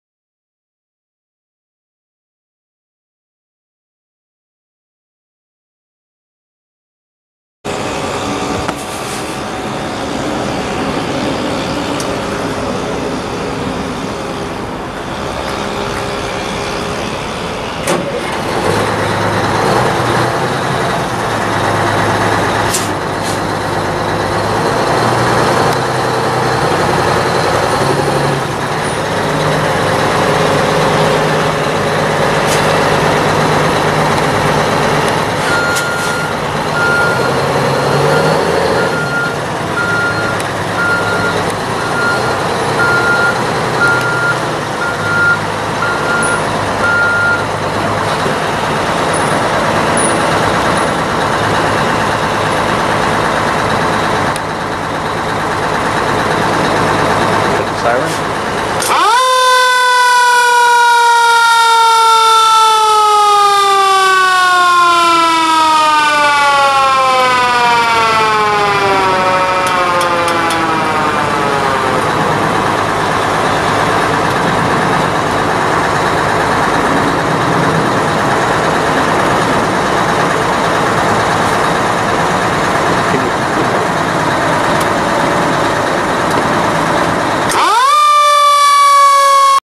1984 Emergency One Pumper firetruck for sale | sold at auction May 3, 2012
SOLD! Colorado Construction Equipment Auction

1984 Emergency One Pumper firetruck
17,730 miles on odometer
8,120 hours on meter
Detroit 6V92 diesel engine
Air brakes
Hale 1,500 gpm pump
Emergency lights and siren
(3) 6" intakes
Front, right, and left side
(6) 2" discharges
(2) right
(2) left
(2) rear
Top swivel discharge
Rear hose reel
Spotlights on bed corners
11R22.5 tires